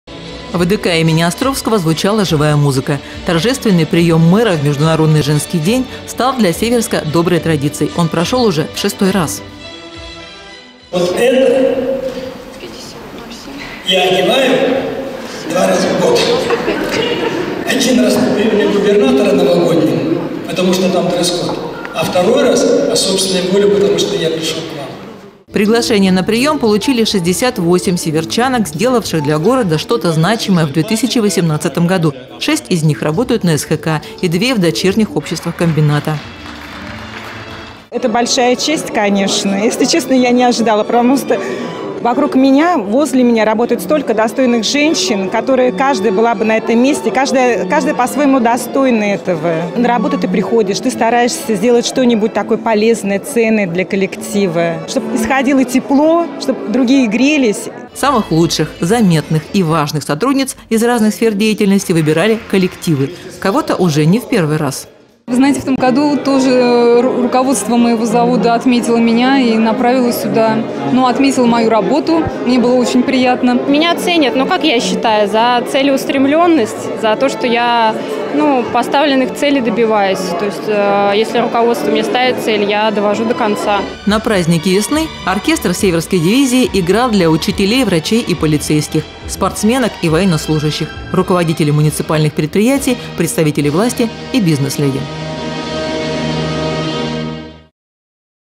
Самые успешные женщины на приёме Мэра Северска
На прием к мэру  северчанки пришли в вечерних нарядах. Поскольку повод был праздничным - 8 марта. Приглашение на прием  получили   68 северчанок, сделавшие  для города что-то значимое в 2018-ом году.  Шесть из  них  работают на  СХК  и две – в дочерних обществах комбината.